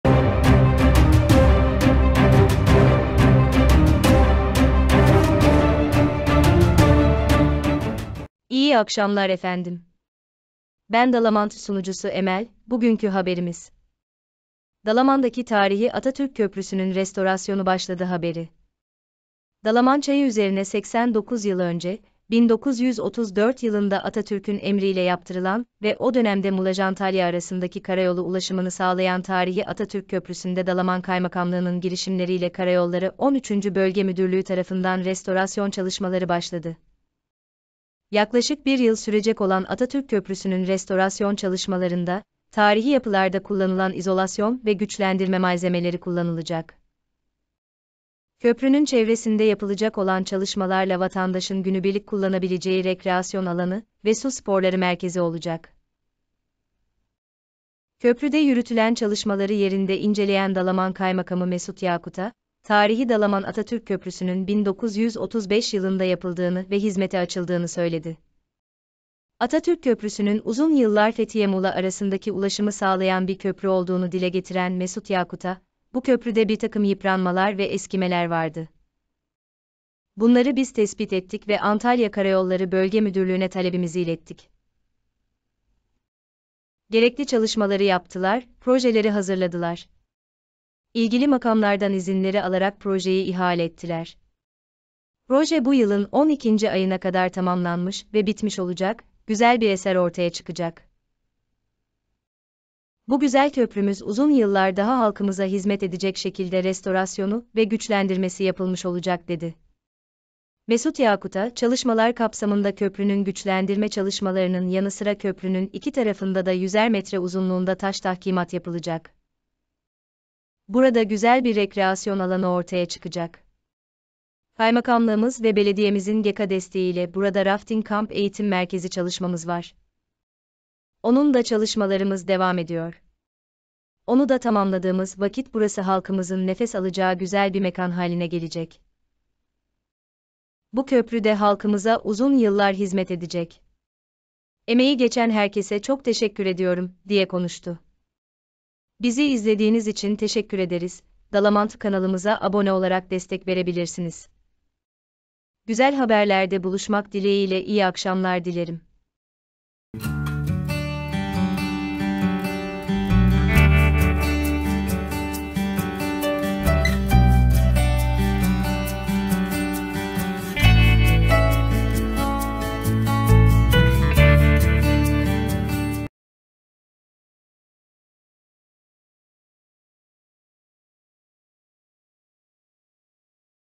DALAMAN DAKİ TARİHİ ATATÜRK KÖPRÜSÜ RESTORASYON ÇALIŞMALARI BAŞLADI.
Dalaman Çayı üzerine 89 yıl önce, 1934 yılında Atatürk’ün emriyle yaptırılan ve o dönemde Muğla-Antalya arasındaki karayolu ulaşımını sağlayan tarihi Atatürk Köprüsünde Dalaman Kaymakamlığı’nın girişimleriyle Karayolları 13. Bölge Müdürlüğü tarafından restorasyon çalışmaları başladı.
Yaklaşık 1 yıl sürecek olan Atatürk köprüsünün restorasyon çalışmalarında, tarihi yapılarda kullanılan izolasyon ve güçlendirme malzemeleri kullanılacak.
Köprünün çevresinde yapılacak olan çalışmalarla vatandaşın günübirlik kullanabileceği rekreasyon alanı ve su sporları merkezi olacak.
Köprüde yürütülen çalışmaları yerinde inceleyen Dalaman Kaymakamı Mesut Yakuta, tarihi Dalaman Atatürk köprüsünün 1935 yılında yapıldığını ve hizmete açıldığını söyledi.
Atatürk köprüsünün uzun yıllar Fethiye-Muğla arasındaki ulaşımı sağlayan bir köprü olduğunu dile getiren Mesut Yakuta, “Bu köprüde birtakım yıpranmalar ve eskimeler vardı. Bunları biz tespit ettik ve Antalya Karayolları Bölge Müdürlüğü’ne talebimizi ilettik. Gerekli çalışmaları yaptılar, projeleri hazırladılar. İlgili makamlardan izinleri alarak projeyi ihale ettiler. Proje bu yılın 12’nci ayına kadar tamamlanmış ve bitmiş olacak, güzel bir eser ortaya çıkacak. Bu güzel köprümüz uzun yıllar daha halkımıza hizmet edecek şekilde restorasyonu ve güçlendirmesi yapılmış olacak” dedi.
Mesut Yakuta, “Çalışmalar kapsamında köprünün güçlendirme çalışmalarının yanı sıra köprünün iki tarafında da yüzer metre uzunluğunda taş tahkimat yapılacak. Burada güzel bir rekreasyon alanı ortaya çıkacak. Kaymakamlığımız ve belediyemizin GEKA desteğiyle burada rafting kamp eğitim merkezi çalışmamız var. Onun da çalışmalarımız devam ediyor. Onu da tamamladığımız vakit burası halkımızın nefes alacağı güzel bir mekan haline gelecek. Bu köprü de halkımıza uzun yıllar hizmet edecek. Emeği geçen herkese çok teşekkür ediyorum” diye konuştu.
-Proje sorumlusu İnşaat Yüksek Mühendisi Mehmet Arslan da, o dönemin dünyadaki köprü tiplerinden esinlenilerek Türk mühendisleri ve Türk bayındırlığının inşa ettiğini söyledi.
Mehmet Arslan, “takriben 80 küsur yıl karayollarımıza hizmet etmiş ve bu sürçte de betonarme taşıyıcı elemanlarında taşıma gücü kayıpları, hava ve iklim şartlarında malzeme deformasyonları, şekil değiştirmeleri, tahribatları olmuş. Karayolları da bu çerçevede 2020 yılında restorasyon güçlendirme projesi yaptırarak 2022’de ihaleye çıktı. Ve biz bunu M-Arslan İnşaat olarak ihaleden aldık, yapımına başladık. Köprünün güçlendirme restorasyonunu mimari yapının mimari süliyetine, mimari koruma kurulu kararına uygun bir şekilde 5-6 ay içerisinde bitirmeye çalışacağız. Sonra dere ıslahı çerçevesinde taş tahkimatına geçeceğiz. Yeniden karayollarının tali bir yolu olarak hizmete açılacak” dedi.
Mehmet Arslan, “Karayolları Genel Müdürlüğü’nün Sanat Yapıları Dairesi Başkanlığı daha önce de Samsun’da ve bu tip köprülerin olduğu bazı yerlerde köprü güçlendirmesini karbon fiber, karbon kanaviçe dediğimiz taşıma gücü yüksek, özellikle çekme gerilmeleri açısından yüksek malzemeyle yapının yapı elemanlarının taşıma gücü kaybını gideren bir projelendirme yapmış. Burada da o proje uygulanacak. Dolayısıyla yeni teknolojik malzemeyle güçlendirme yapılıp, ancak mimari tarihseli ve mimari restorasyon yapının yapı elemanlarının mimari özgün özelliği korunacak. Dolayısıyla biz restorasyon güçlendirmesini bitirdiğimizde mimari yönden yapı orijinalliğini, tabiliğini korumuş olacak. Ama en az bir yüzyıl daha taşıma gücü ve yapı statiği açısından, yapı mukavemeti açısından yapı güçlendirilmiş olacak” ifadelerini kullandı.
-Ege ile Akdeniz’i birbirine bağlayan tarihi Atatürk köprüsü, Atatürk’ün emri, Başbakan İsmet İnönü’nün Nafia Vekili (Bayındırlık Bakanı) Ali Çetinkaya’ya talimatıyla yaptırıldı. O dönemde Muğla-Antalya arasındaki karayolu ulaşımını sağlayan tarihi Atatürk Köprüsünün yapımına 1934 yılında başlandı. Atatürk Köprüsü, 1935 yılında 170 işçi tarafından 1 yılda Fransız mimarisine uygun olarak, bombeli kemer şeklinde inşa edildi. 4 metre genişliğe, 14 metre yüksekliğine sahip, 3 ayak üzerine kurulan köprü 117 metre uzunluğunda.